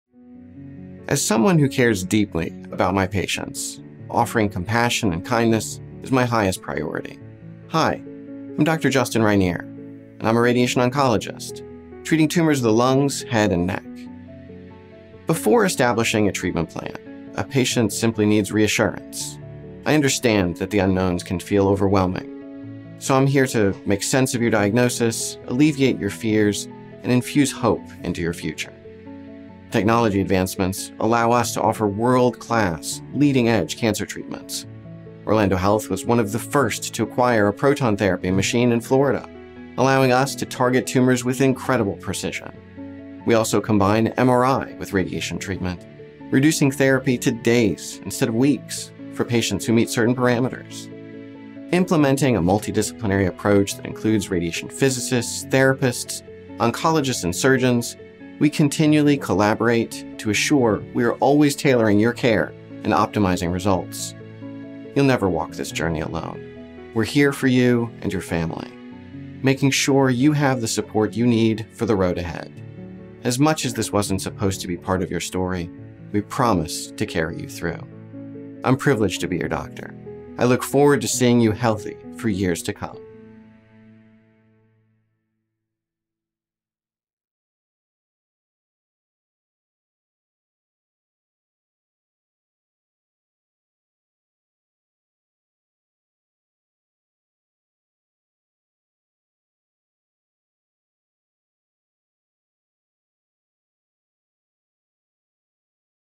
Justin Rineer, MD
Get to know Dr. Justin Rineer, a board-certified radiation oncologist, specializing in proton therapy, with Orlando Health Cancer Institute.